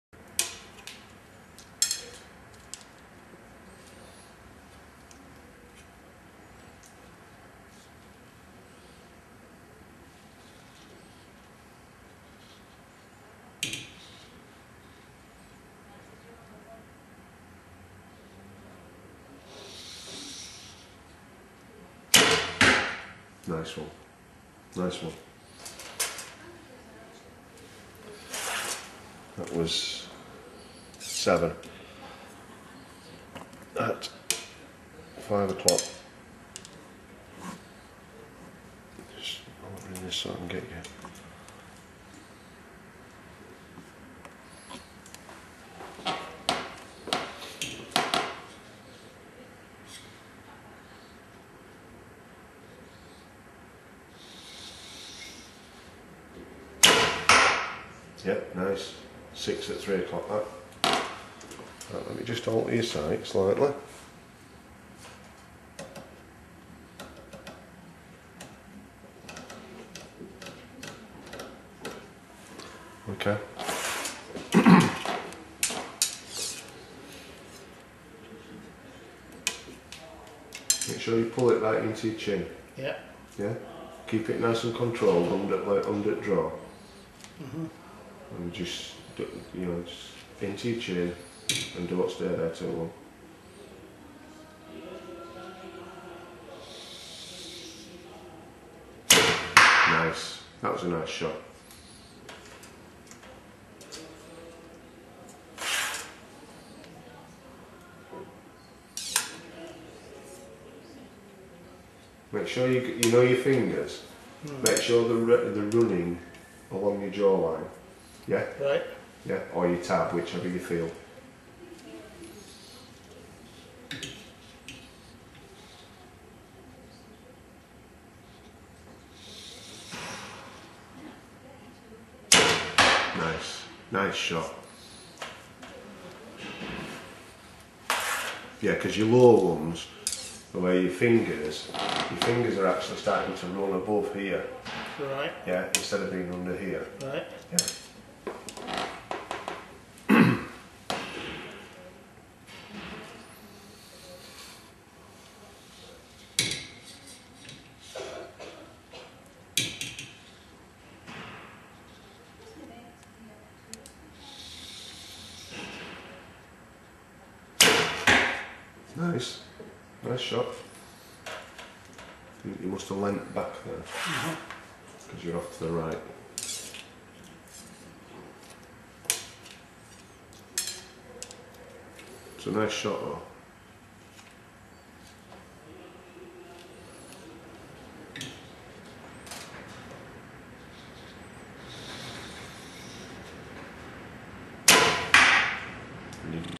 Harry The Visually Impaired Archer 2007
Coaching session with Harry. Getting Harry back into the swing of things again after his break, through illness.